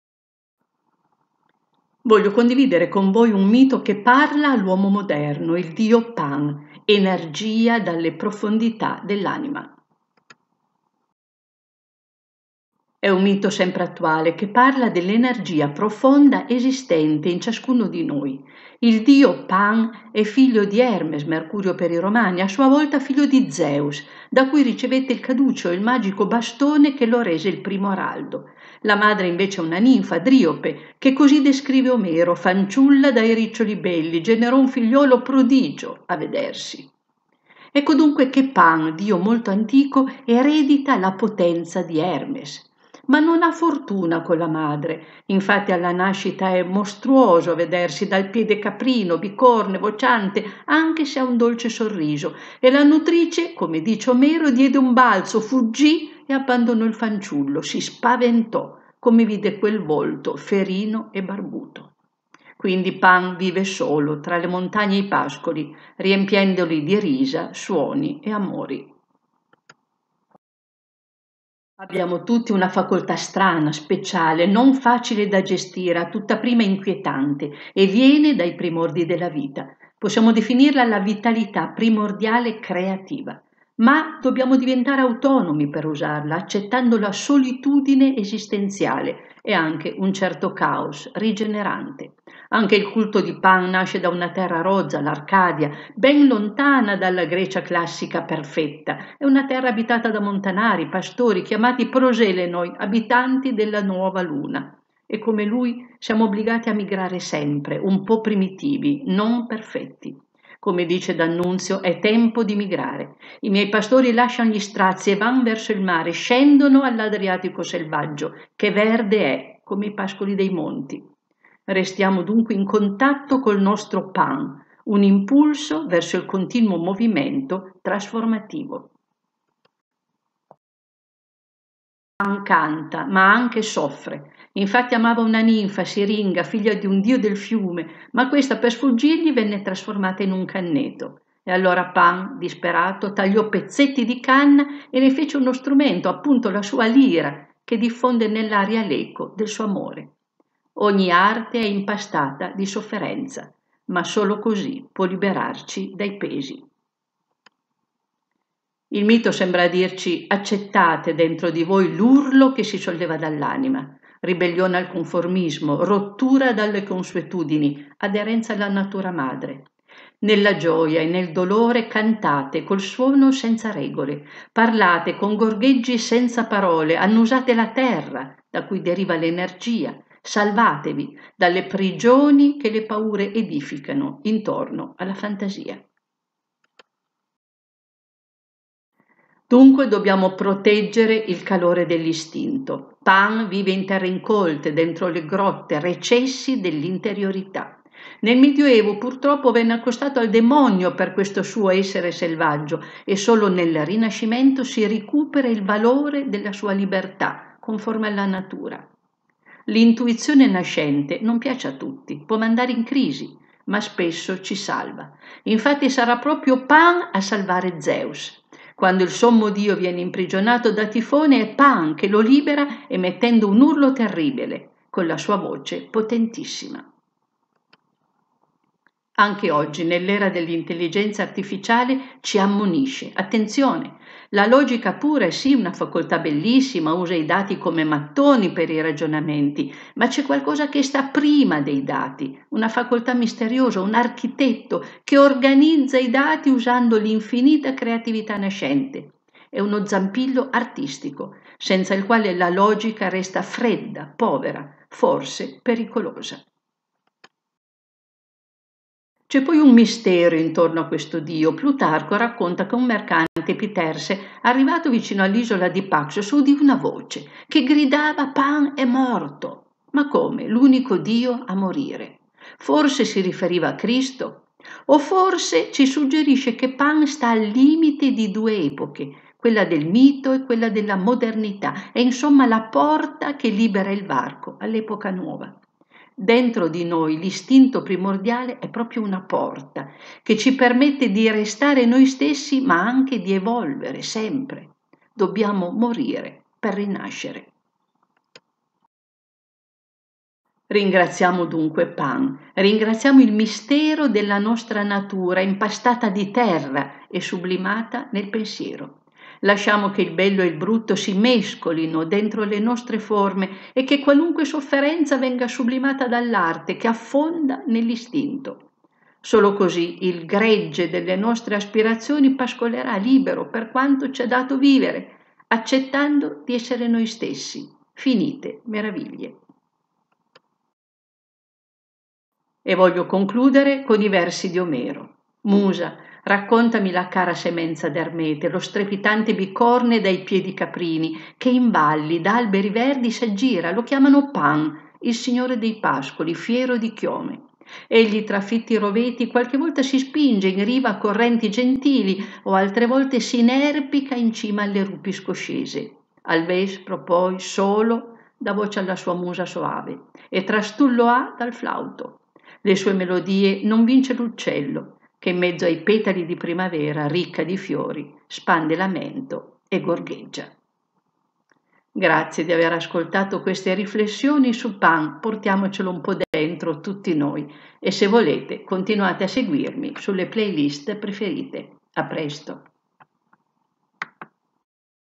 Un mito che parla all'uomo moderno. Il dio Pan. Energia che viene dal profondo.
Questo Dio personifica la forza nascente legata alla natura. Ci ammonisce a tener vivo dentro di noi il contatto con l'energia primordiale, per permetterci un'evoluzione trasformativa.